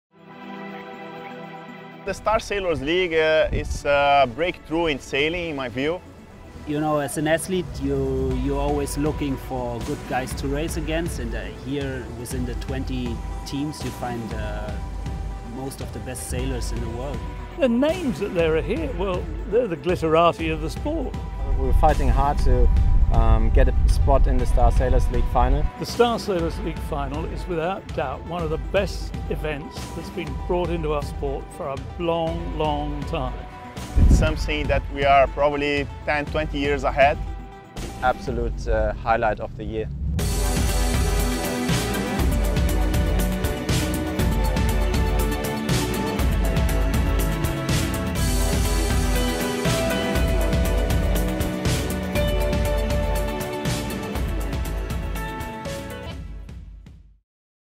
Star Sailors League Finals - The Trailer
The Star Sailors League Finals are the climax of the SSL Circuit and crown the winner of 4 intense days of sailing in an exciting and innovative racing format.

Some of the greatest names of the sailing sport took part in 2014 as Torben Grael, Robert Scheidt, Fredrik Lööf, Xavier Rohart, Mateusz Kusznierewicz, Mark Mendelblatt, Giles Scott and many others, all fighting to get the bigger bite to the overall prize money of 200,000 US$.